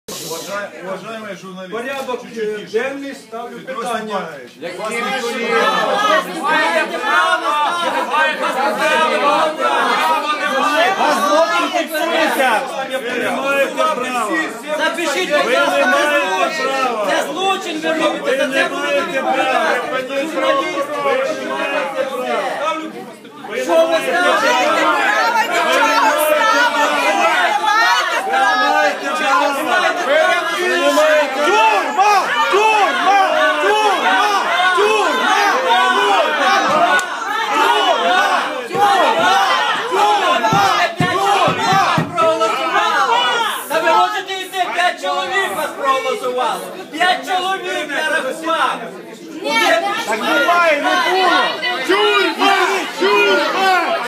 На окрузі Іллєнка пропонують оголосити перерву
На 215 окрузі в Києві, де ведуть боротьбу самовисуванець, секретар Київміськради Галина Герега та кандидат від ВО «Свобода» Андрій Іллєнко,  заступник голови ОВК пропонує оголосити перерву в засіданні до 14:00 31 жовтня.